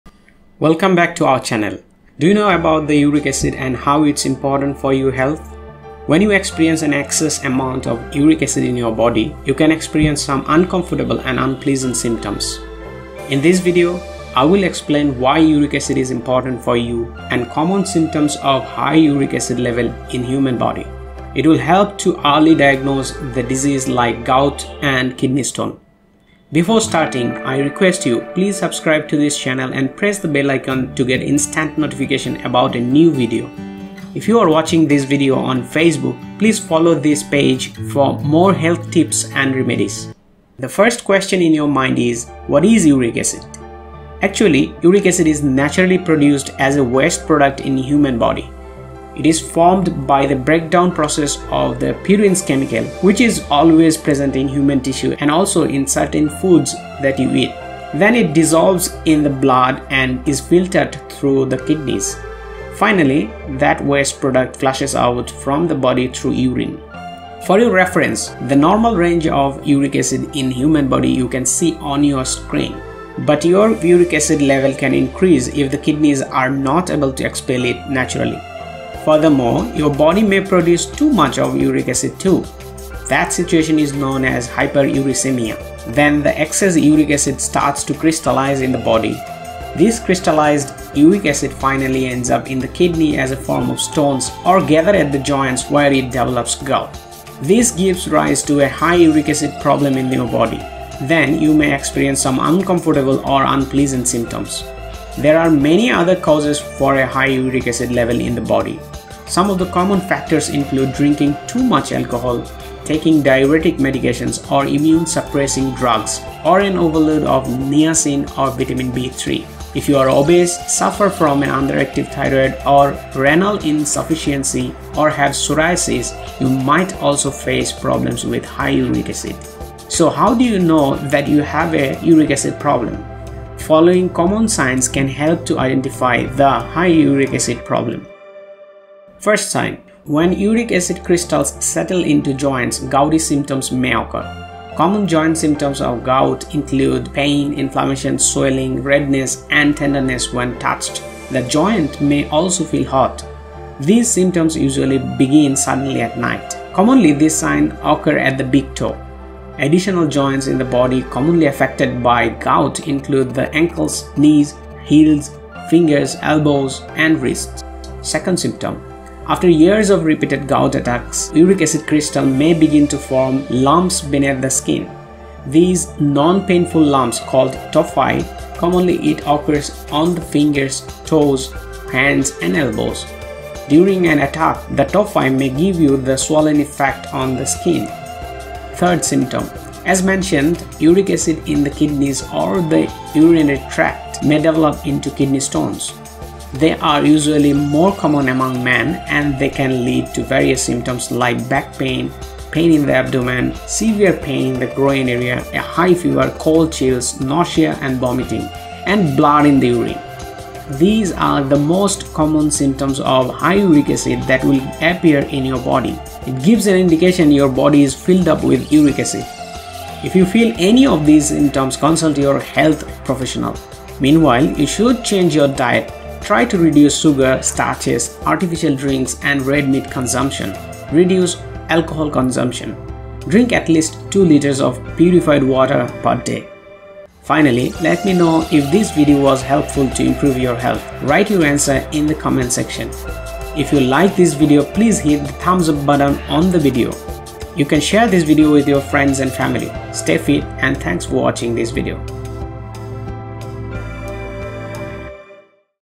Signs and symptoms of high uric acid level in a body
Know the various symptoms that show a person has high uric acid levels in his body. It occurs due to when the kidney doesn't clear the excess purines from a body.

Uric is a naturally produced waste product in the system. These common signs help to early diagnose the disease like gout and kidney stone.